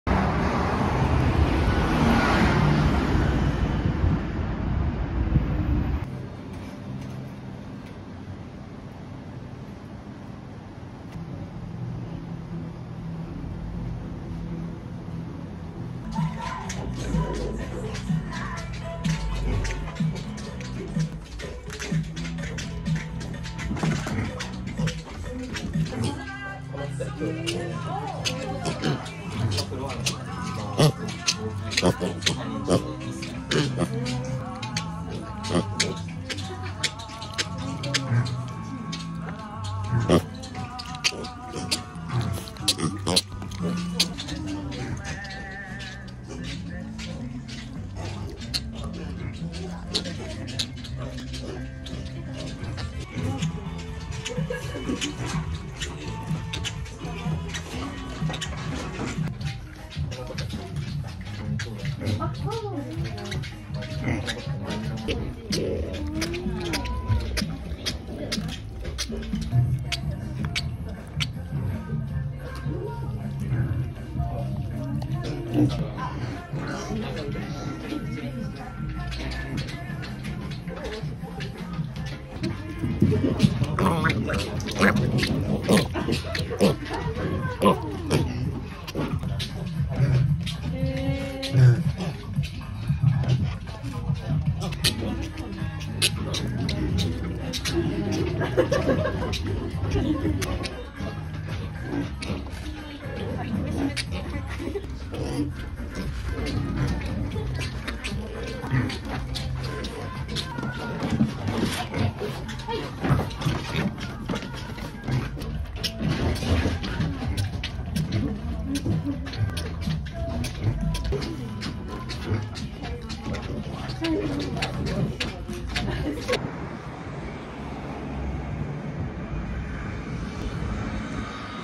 東京迷你豬咖啡 mipig cafe 目黑店 mini micro pig 日本 japan tokyo
店名:mipig cafe 目黑店
地址:東京都目黑區目黑4-11-3
最近地鐵站:目黑站
最近公車站:元競馬場前